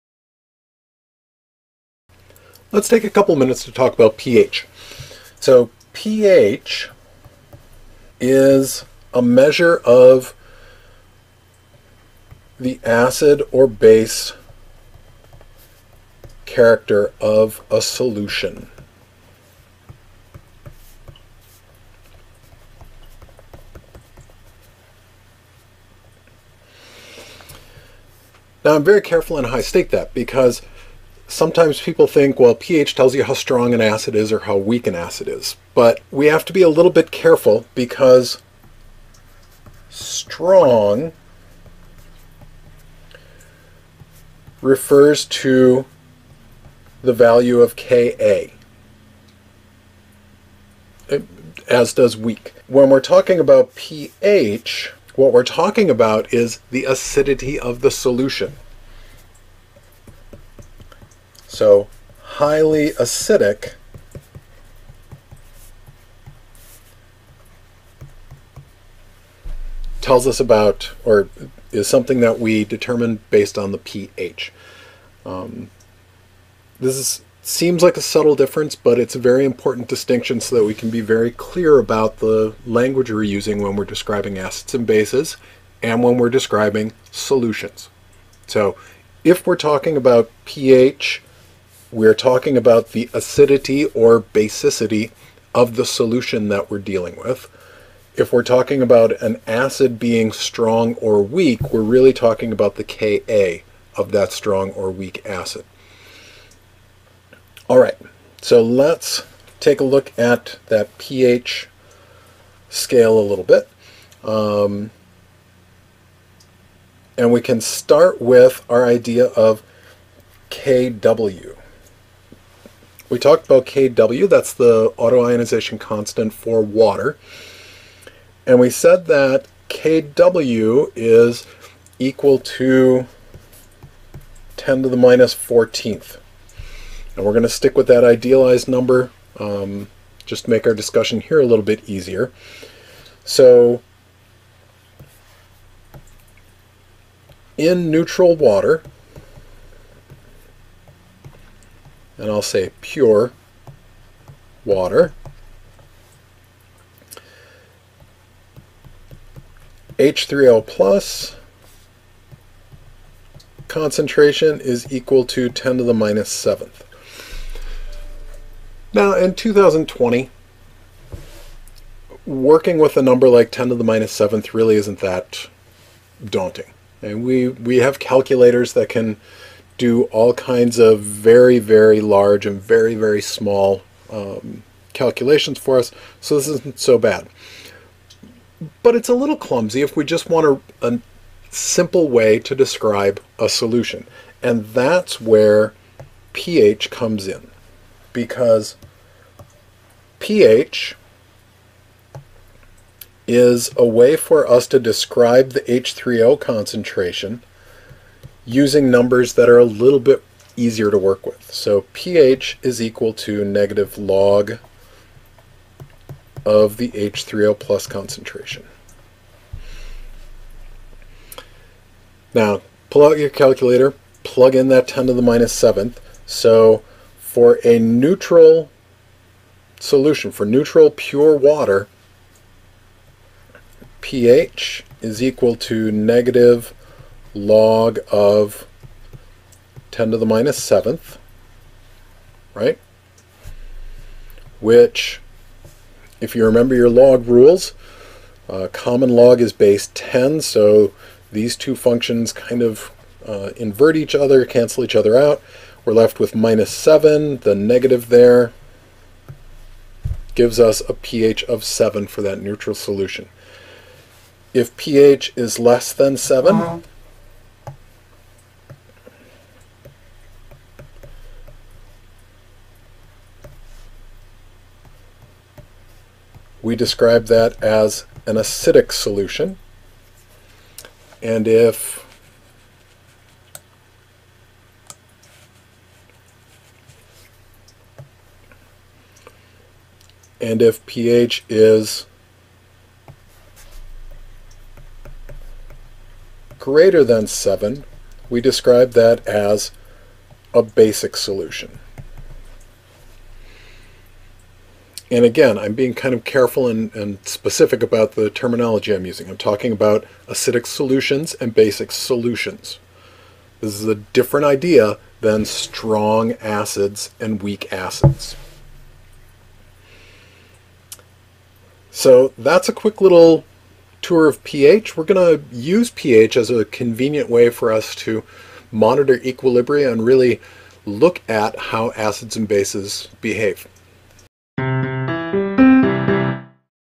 pH - Is it an acid or a base?
The chemistry of acids and bases affects every aspect of our daily lives. We can use pH to measure how acidic or basic a solution is and gain some insight into the chemical behaviour of water in the process. (Gen Chem Level)